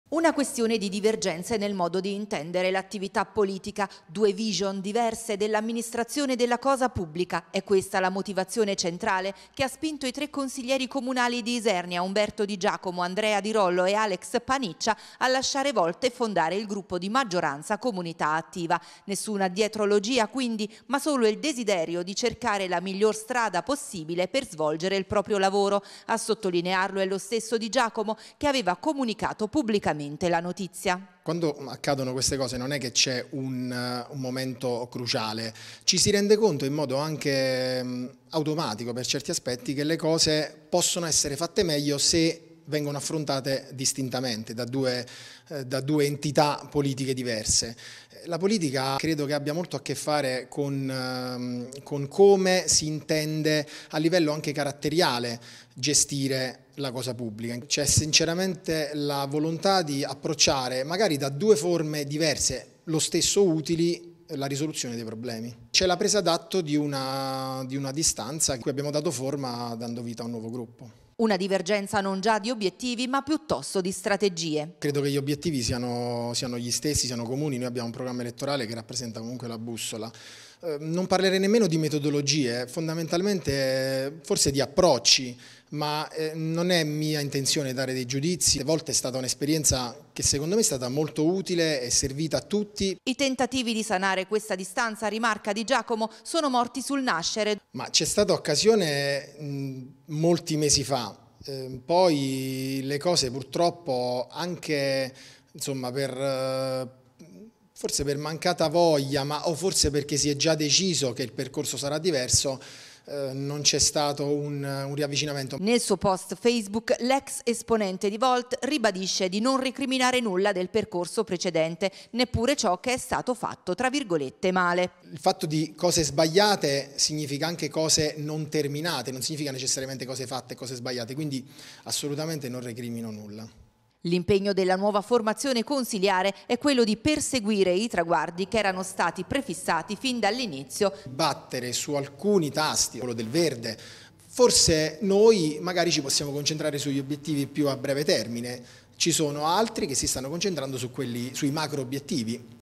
𝗩𝗢𝗟𝗧, 𝗗𝗜 𝗚𝗜𝗔𝗖𝗢𝗠𝗢: 𝗔𝗣𝗣𝗥𝗢𝗖𝗖𝗜 𝗗𝗜𝗩𝗘𝗥𝗚𝗘𝗡𝗧𝗜
Via da Volt per una divergenza di approcci alla politica e all’amministrazione della cosa pubblica